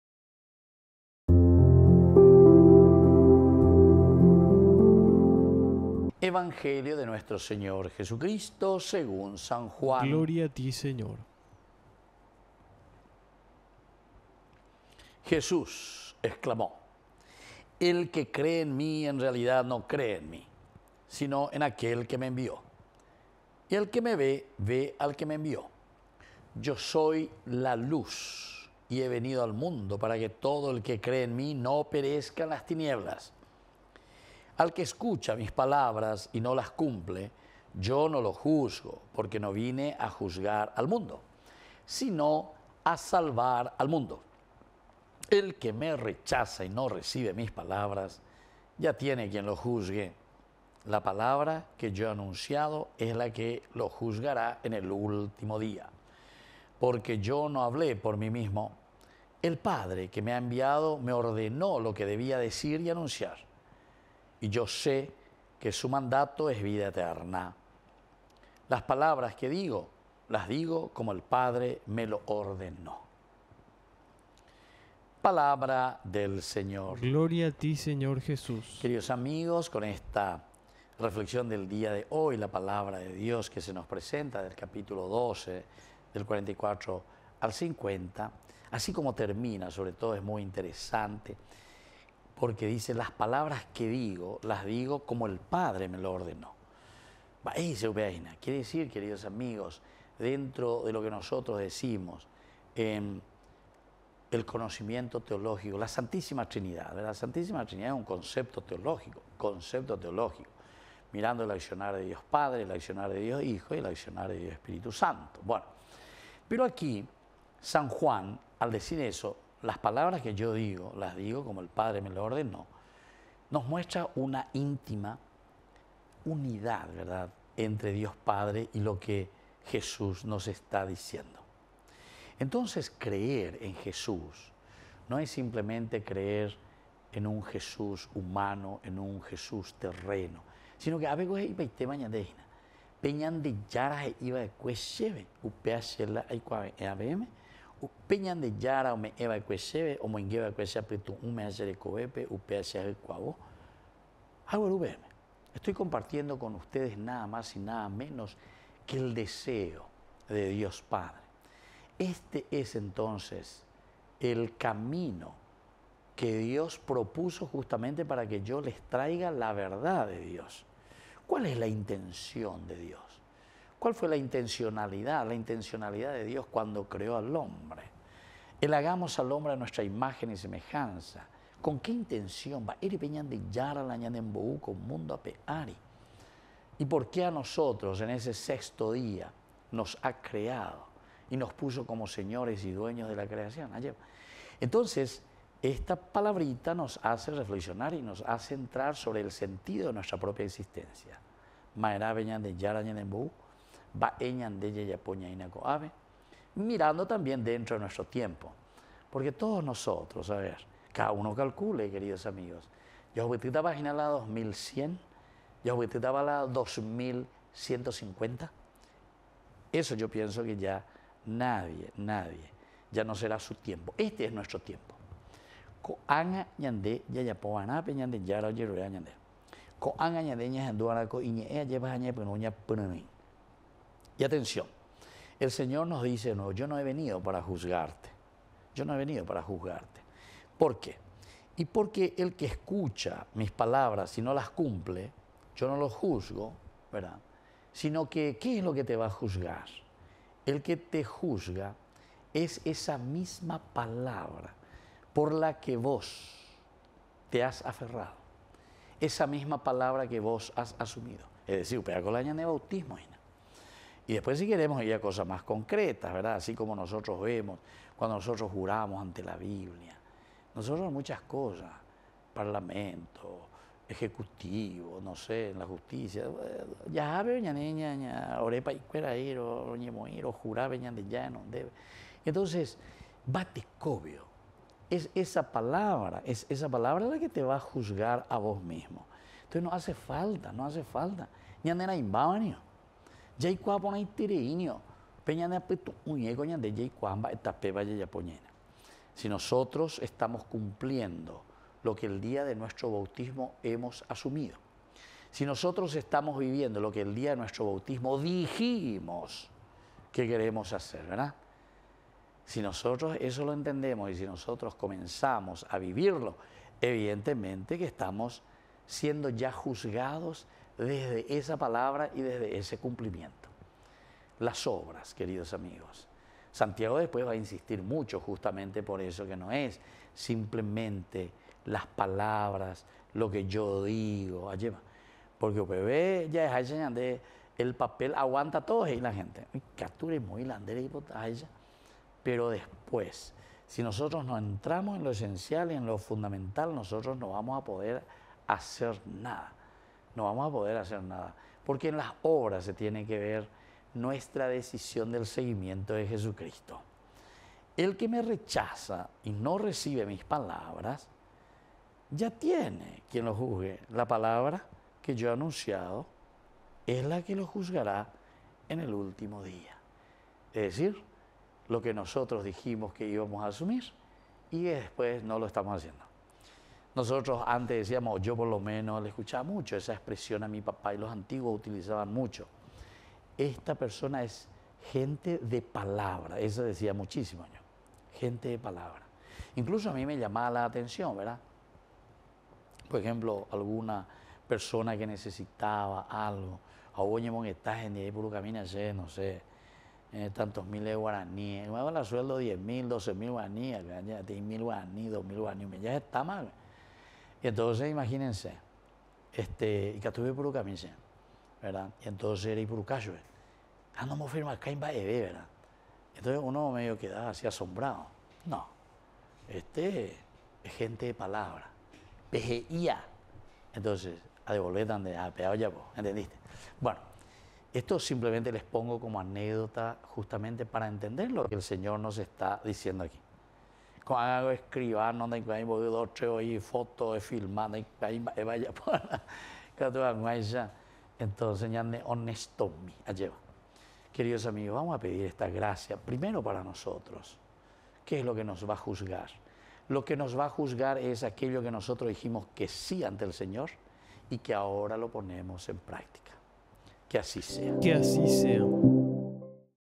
HOMILIA DE HOY | Miércoles 24 de Abril del 2024
"Jesús declaró públicamente: El que cree en mí no cree en mí, sino en el que me ha enviado, y el que me ve, ve al que me ha enviado. Yo, la luz, he venido al mundo, para que quien crea en mí no tenga que permanecer más en la oscuridad."